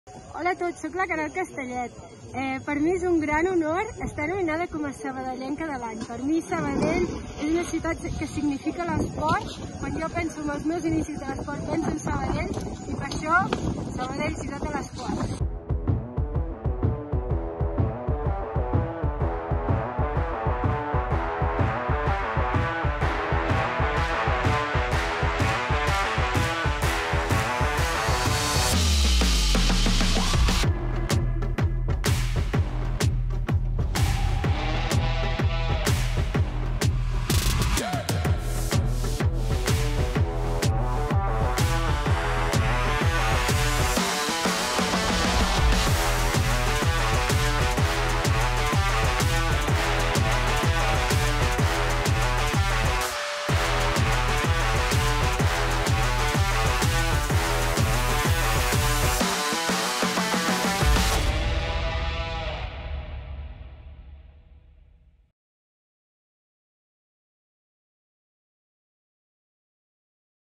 Queralt Castellet
L'esportista sabadellenca Queralt Castellet es va convertir als Jocs d'Hivern de Pequín 2022, els seus cinquens, en la primera catalana en ser medallista en unes Olimpíades d'hivern.